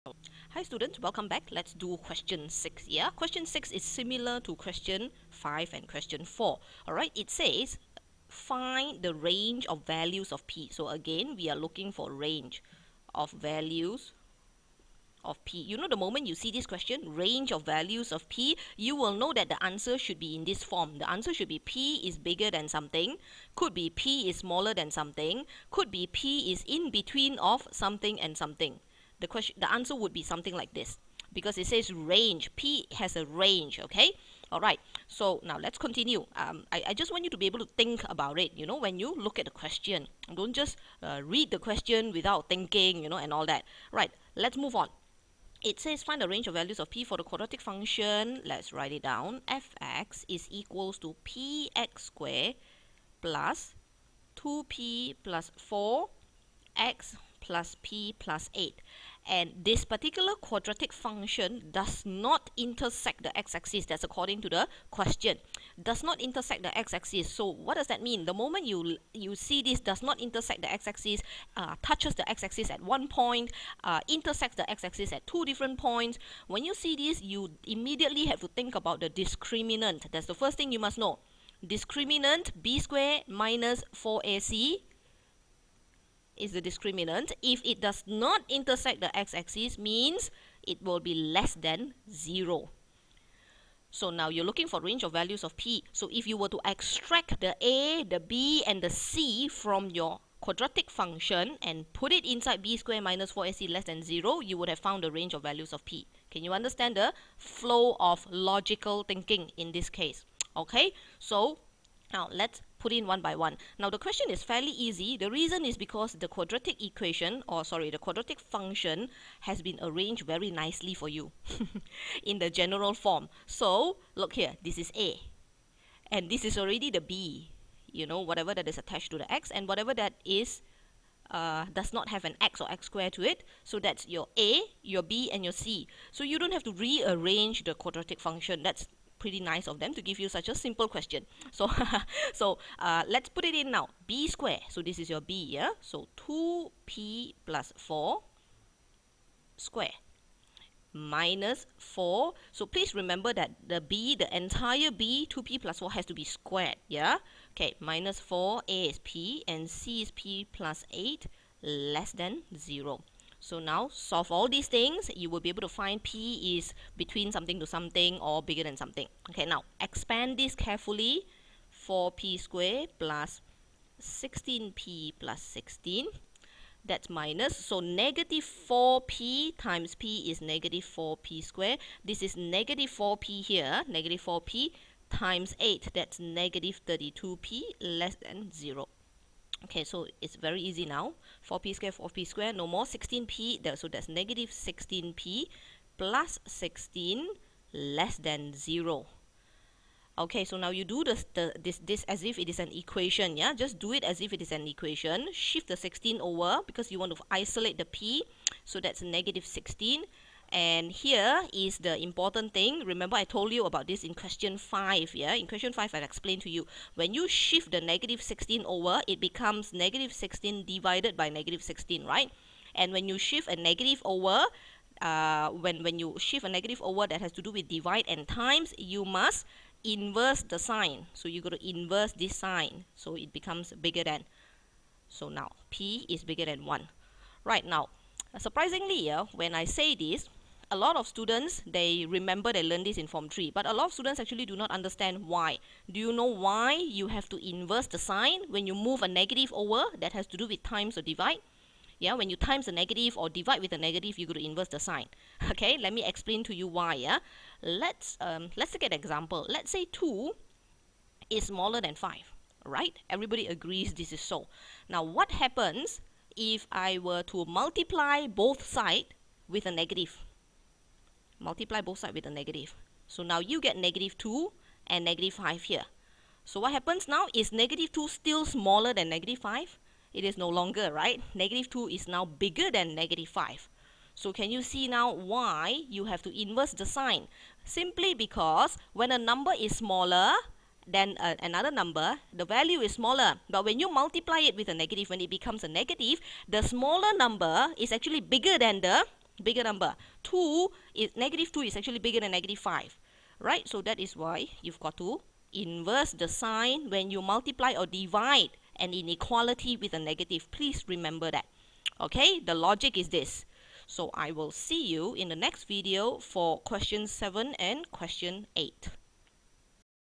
SPM Add Math Quadratic Functions - discriminant
How to use the Discriminant in Quadratic Functions SPM Add Math Form 4